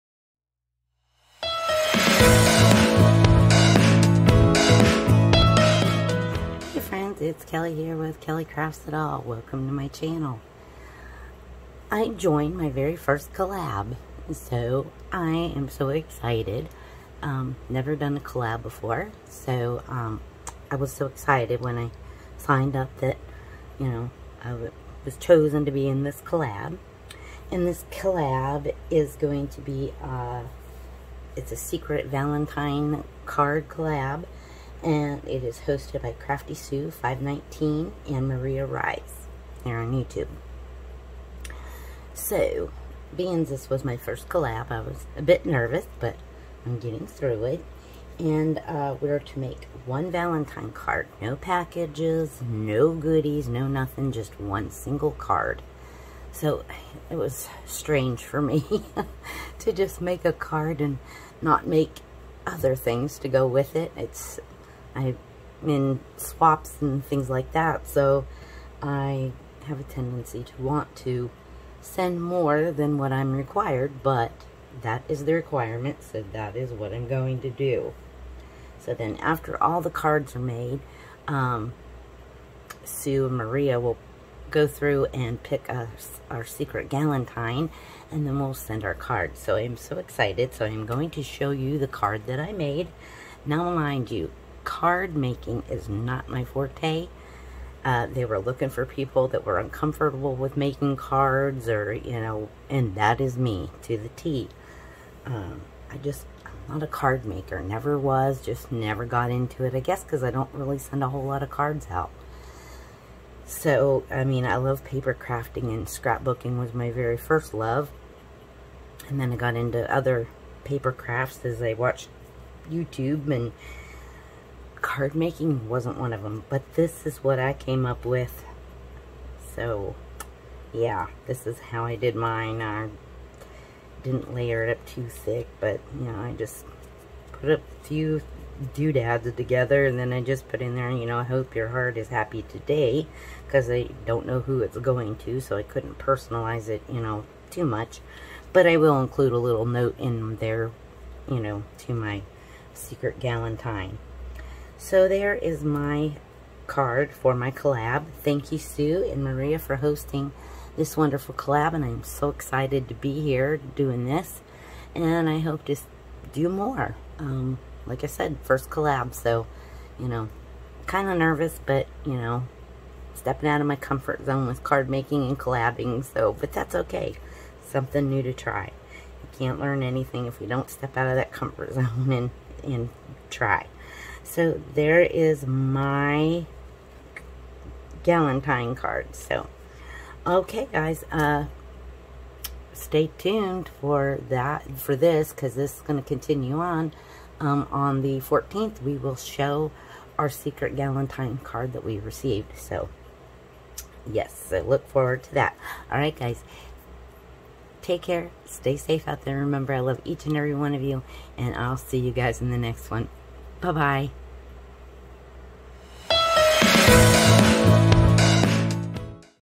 Secret Valentine Collab
A secret Valentine collab hosted by:
CraftySue519 & Maria Reyes.
I am so excited to be in my first collab!
Thank you ladies for hosting a wonderful collab.
Hugs
Follow these amazing channels to be inspired to make great
Valentine cards.